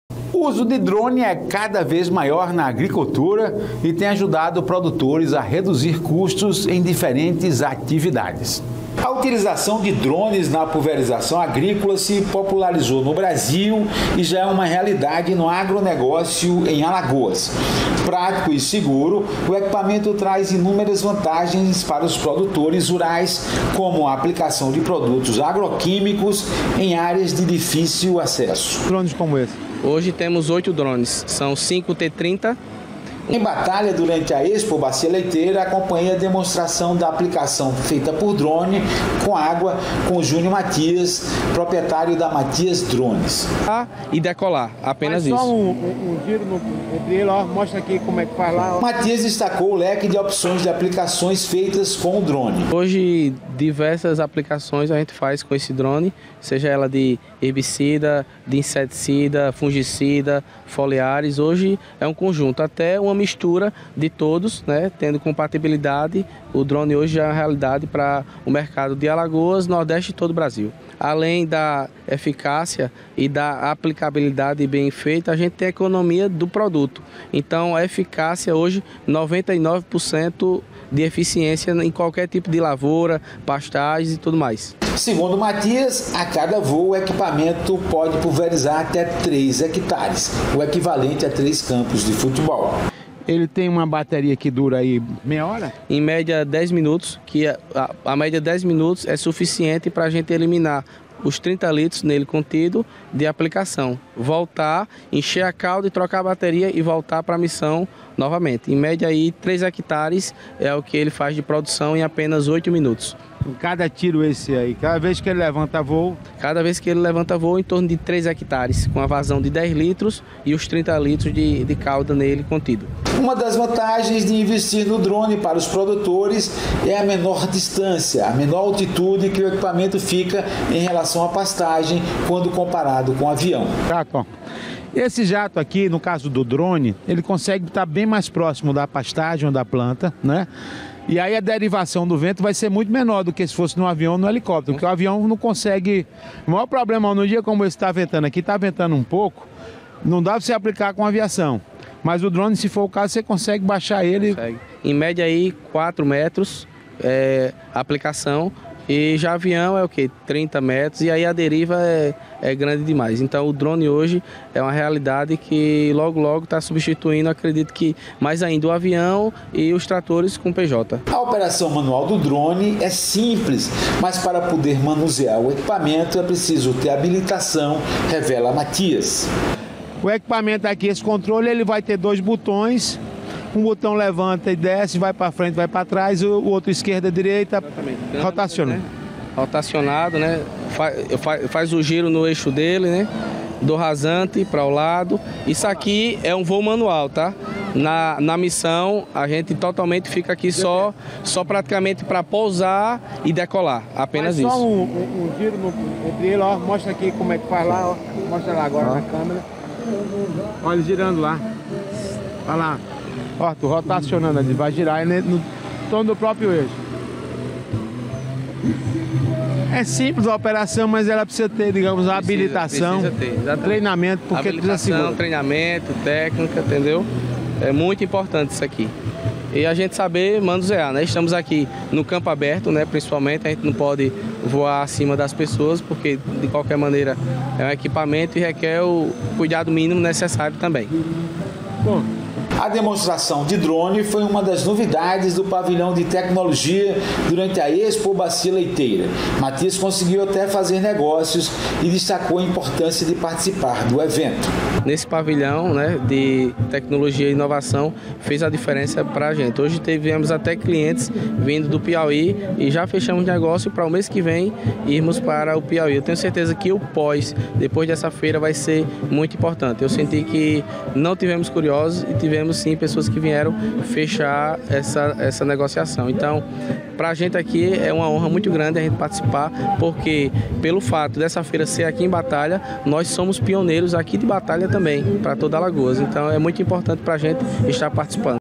Uso de Drones é cada vez maior na agricultura
O equipamento traz inúmeras vantagens para os produtores rurais.